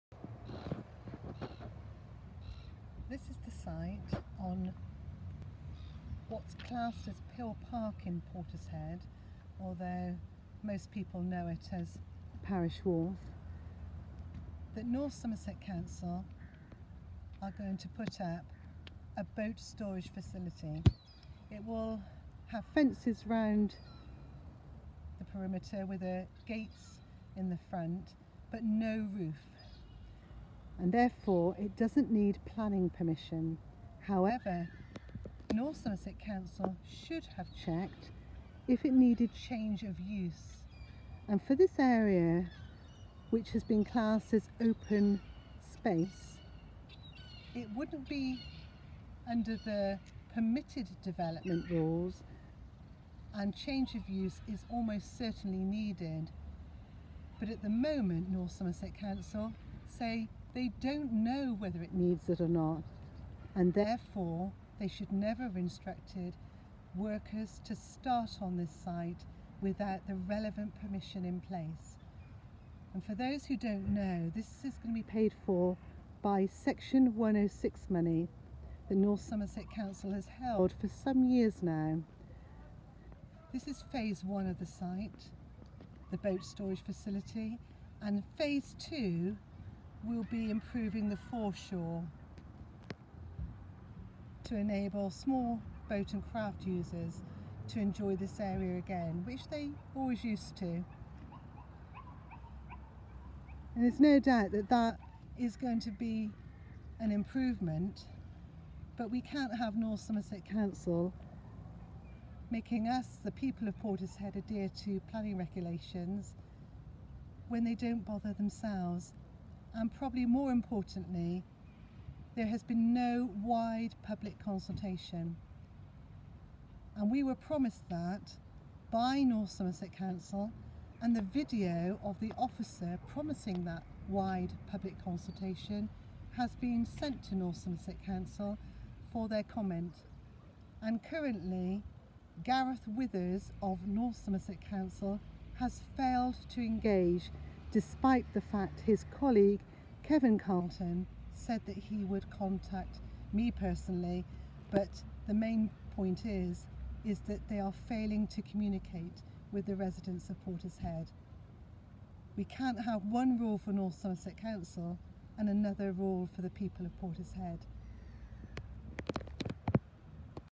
Pill Park Boat Storage Facility For Which North Somerset Council Has Not Applied for Change of Use
The last Planning Application for a boat house on Pill Park/Parish Wharf land using Portishead’s valuable Section 106 funding, expired in 2017 after five years of inactivity by the applicant North Somerset Council (NSC).  The recent works on this land for a new boat storage facility that will have a fenced surround and gated front with no roof, NSC says will not need planning permission.  However, it is likely to need Change of Use permission and although NSC has already instructed Works to commence at the site, North Somerset Council failed to do two things.
1. Undertake a “wide public consultation” as promised by NSC officers including a display within Parish Wharf Leisure Centre to inform and engage with the people of Portishead.
 2. Ensure that investigations were carried out as to whether Change of Use was required BEFORE Works commenced.
It seems that NSC does not think that the rules which apply to the people of Portishead with regard to planning matters, applies to the district council.  There is one rule for us and another for NSC. It may be their land, but they have to follow due process. So next time David Pasley, before running to the press with quotes to improve your failing reputation,  check, as the NSC Executive Member, that your Council has done ALL the paperwork and fully engaged with the wider public.